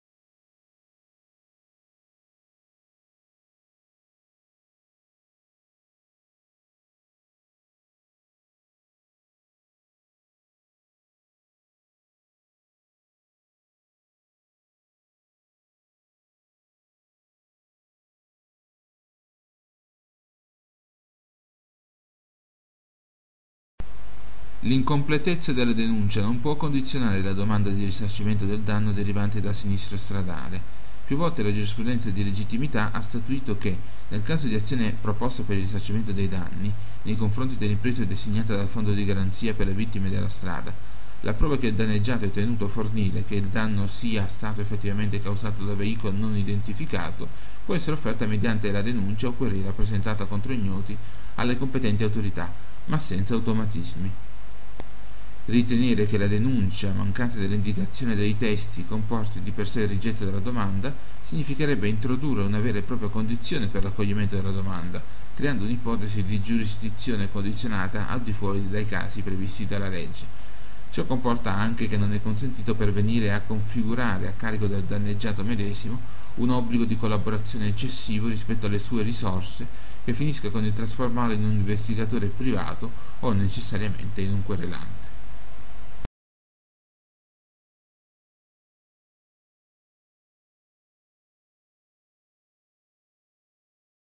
Auto pirata e sinistro: Risarcimento anche senza testimoni
Auto pirata e sinistro: Risarcimento anche senza testimoni  -- Hit-and left: Compensation without witnesses
Cassazione Civile, Sezione Terza, sentenza del 18 giugno 2012, n. 9939

L'incompletezza della denuncia non può condizionare la domanda di risarcimento del danno derivante da sinistro stradale.
Più volte la giurisprudenza di legittimità ha statuito che, in caso di azione proposta per il risarcimento dei danni, nei confronti dell'impresa designata dal Fondo di Garanzia per le vittime della strada, la prova che il danneggiato è tenuto a fornire che il danno sia stato effettivamente causato da veicolo non identificato, può essere offerta mediante la denuncia o querela presentata contro ignoti alle competenti autorità, ma senza automatismi.
Ritenere che la denunzia mancante dell'indicazione dei testi comporti, di per sé, il rigetto della domanda, significherebbe introdurre una vera e propria condizione per l'accoglimento della domanda, creando un'ipotesi di giurisdizione condizionata al di fuori dai casi previsti dalla legge.
Ciò comporta anche che non è consentito pervenire a configurare a carico del danneggiato medesimo un obbligo di collaborazione eccessivo rispetto alle sue "risorse, che finisca con il trasformarlo in un investigatore privato o necessariamente in un querelante.

ENG
The incompleteness of the complaint can not influence the application for compensation for damage caused by traffic accident.
Several times the jurisprudence of legitimacy held that, if the action is proposed for the damages, against the company designated by the Guarantee Fund for the victims of the road, proof that the claimant must provide that the damage was actually caused by an unidentified vehicle, may be offered by the complaint or lawsuit filed against unknown to the authorities, but without automation.
Believe that the complaint without providing the text implies, in itself, the refusal of the application would introduce a real condition for the acceptance of demand, creating an assumption of jurisdiction by conditioning on the outside as required by law .
This also means that it is not allowed to even say that the injured party has an obligation to cooperate that is excessive in relation to its "resources, which in turn ends up with a private investigator or necessarily a plaintiff."

FRE
Le caractère incomplet de la plainte ne peut pas influencer la demande d'indemnisation pour les dommages causés par accident de la circulation.
Plusieurs fois la jurisprudence de la légitimité de constater que, si l'action est proposé pour les dommages et intérêts contre la société désignée par le Fonds de garantie pour les victimes de la route, la preuve que le demandeur doit fournir que les dommages a été effectivement causé par un véhicule non identifié, peut être offert par la plainte ou action en justice intentée contre inconnu des autorités, mais sans automatisation.
Croire que la plainte sans fournir le texte implique, en soi, le refus de la demande serait d'introduire une condition réelle pour l'acceptation de la demande, la création d'une hypothèse de compétence par le conditionnement à l'extérieur tel que requis par la loi .
Cela signifie aussi qu'il n'est pas autorisé à dire que même la partie lésée a l'obligation de coopérer qui est excessif par rapport à ses «ressources, qui à son tour se termine par un enquêteur privé ou nécessairement un demandeur."

ESP
El carácter incompleto de la queja no puede influir en la solicitud de indemnización por daños causados ​​por accidente de tráfico.
En varias ocasiones la jurisprudencia de la legitimidad sostuvo que, si la acción se propone para los daños y perjuicios, en contra de la empresa designada por el Fondo de Garantía para las víctimas de la carretera, prueba de que el demandante debe establecer que el daño fue causada en realidad por un vehículo no identificado, pueden ser ofrecidos por la queja o demanda contra desconocido para las autoridades, pero sin la automatización.
Cree que la denuncia sin proporcionar el texto implica, en sí mismo, la denegación de la solicitud sería introducir una verdadera condición para la aceptación de la demanda, la creación de una asunción de competencias por el condicionamiento en el exterior como lo exige la ley .
Esto también significa que no se le permite decir incluso que el perjudicado tiene la obligación de cooperar que es excesivo en relación con sus recursos ", que a su vez termina con un investigador privado o necesariamente un actor."